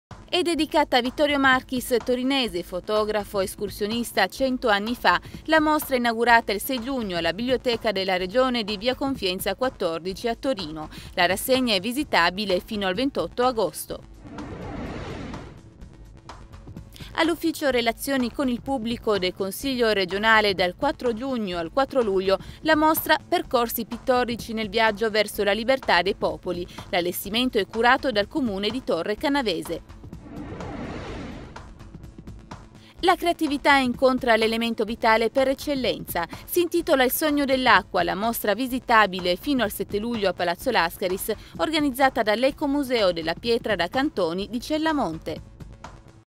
CrpTv n.19 del 15 giugno 2012 - Mostre in corso
Vittorio Marchis torinese, fotografo, escursionista 100 anni fa
È dedicata a Vittorio Marchis torinese, fotografo escursionista 100 anni fa la mostra inaugurata il 6 giugno alla Biblioteca della Regione di via Confienza 14, a Torino. La rassegna è visibile fino al 28 agosto.

Percorsi pittorici nel viaggio verso la libertà dei popoli
All'Ufficio relazioni con il pubblico del Consiglio regionale, dal 4 giugno al 4 luglio, la mostra Percorsi pittorici nel viaggio verso la libertà dei popoli. L'allestimento è curato dal Comune di Torre Canavese.

Il sogno dell'acqua
La creatività incontra l'elemento vitale per eccellenza. Si intitola "Il sogno dell'acqua", la mostra visitabile fino al 7 luglio a Palazzo Lascaris, organizzata dall'Ecomuseo della pietra da cantoni di Cella Monte.